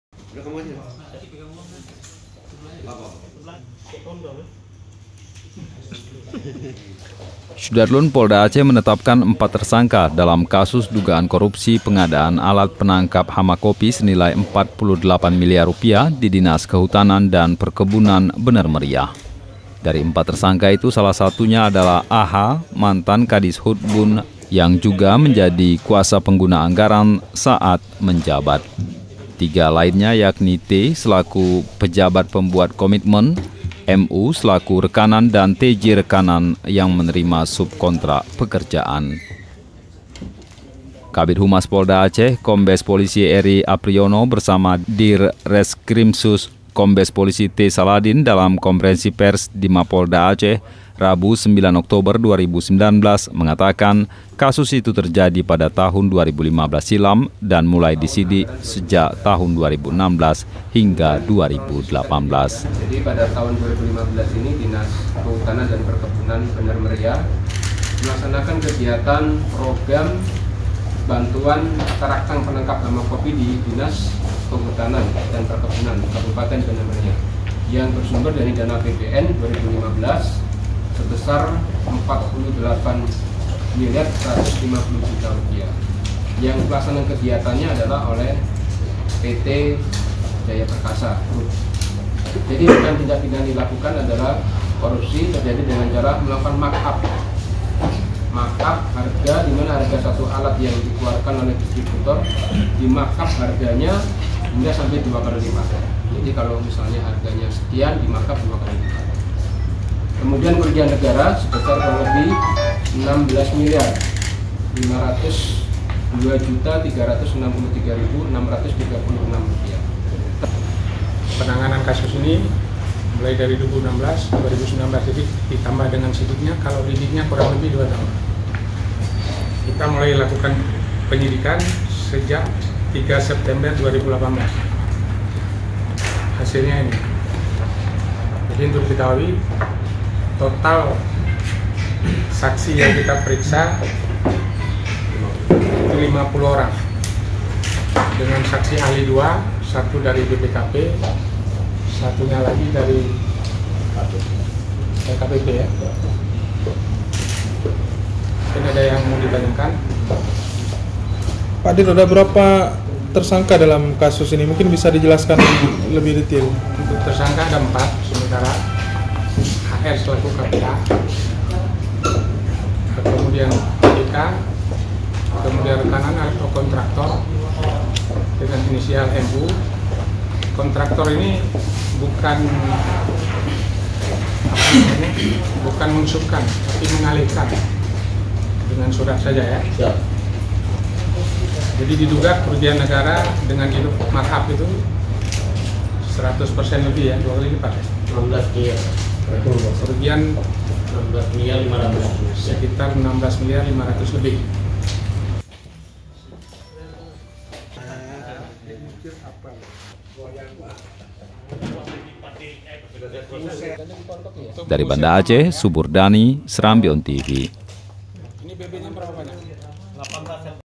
Korupsi Pengadaan Alat Penangkap Hama Kopi, Polda Tetapkan 4 Tersangka
SERAMBITV.COM, BANDA ACEH - Polda Aceh menetapkan empat tersangka dalam kasus dugaan korupsi pengadaan alat penangkap hama kopi senilai Rp 48 miliar di Dinas Kehutanan dan Perkebunan (Dishutbun) Bener Meriah.

Dari empat tersangka itu, salah satunya adalah AH, mantan Kadishutbun yang juga menjadi kuasa pengguna anggaran (KPA) saat menjabat.

Tiga lainnya yakni T selaku pejabat pembuat komitmen (PPK), MU selaku rekanan, dan TJ rekanan yang menerima sub kontrak pekerjaan.

Kabid Humas Polda Aceh, Kombes Pol Ery Apriyono bersama Dir Reskrimsus, Kombes Pol T Saladin dalam konferensi pers di Mapolda Aceh, Rabu (9/10) mengatakan, kasus itu terjadi pada tahun 2015 silam dan mulai disidik sejak tahun 2016 hingga 2018.

Keempat tersangka kini masih berada di Bener Meriah dan akan diserahkan ke Kejaksaan Negeri Bener Meriah Kamis 10 Oktober.

Menurut Ery, korupsi dilakukan oleh para tersangka dengan cara melakukan mark-up atau penggelembungan harga alat perangkap hama kopi tersebut.

Akibat dari perbuatan itu, menurut Kabid Humas, kerugian negara dalam kasus tersebut mencapai Rp 16 miliar lebih.